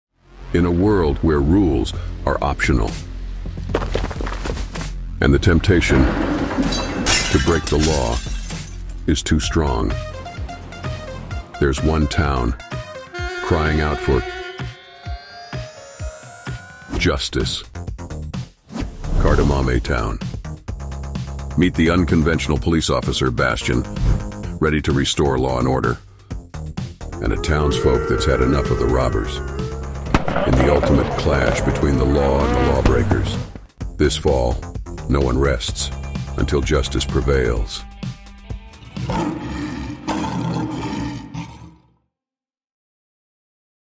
Kardemomme Town | Official Trailer
The trailer for the new version of "Folk og Røvere i Kardemomme By". Based on the visionary book by Thorbjørn Egner. In the film we are set in the lawless town of Kardemomme, where the only rule that exist is that you should not bother other, be nice, and then you can do whatever you want.

This leads to the anarchistic chaos, mostly led by the three robbers Casper, Jesper and Jonathan. The unconventional police officer Bastian tries to cope with all the crime, which leads to this action packed story. Sit back and enjoy the ride. It will be bumpy!

This fake trailer is made with the help of various AI services, such as Midjourney, Photoshop (generative fill), ChatGPT and ElevenLabs.